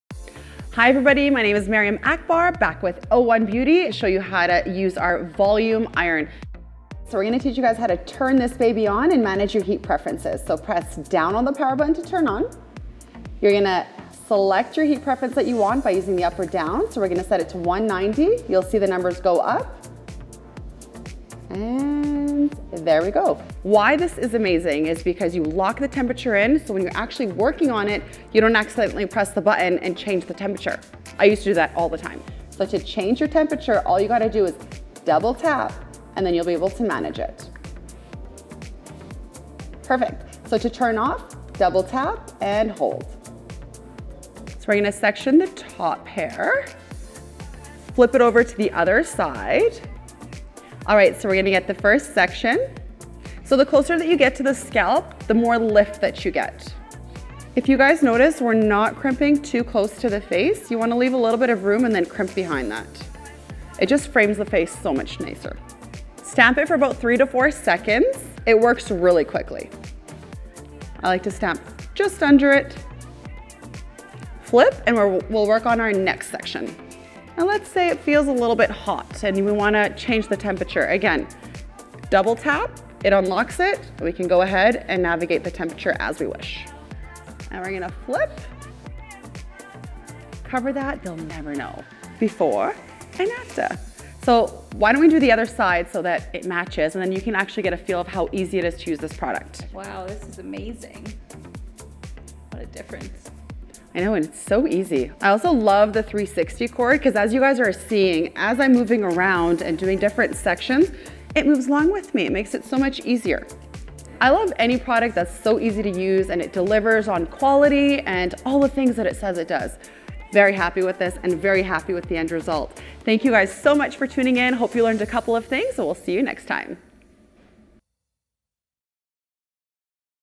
o1beauty - Volume Iron Tutorial (o1brand)
Our Volume Iron is quick & easy to use to give your hair the volume it needs. Please enjoy our tutorial video.

🠗🠗  Available for purchase on our website and on Amazon!   🠗🠗